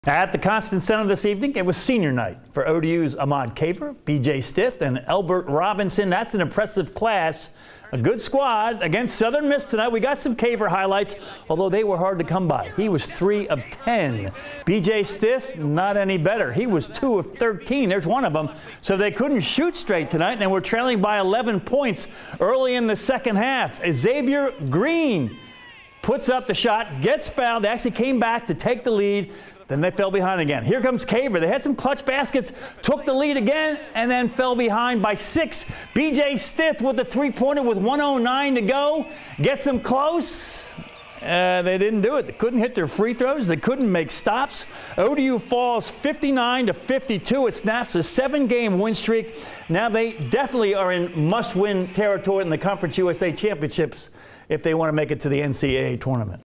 ODU upset on Senior Night
Southern Miss snapped ODU's 7 game win streak by beating the Monarchs 59-52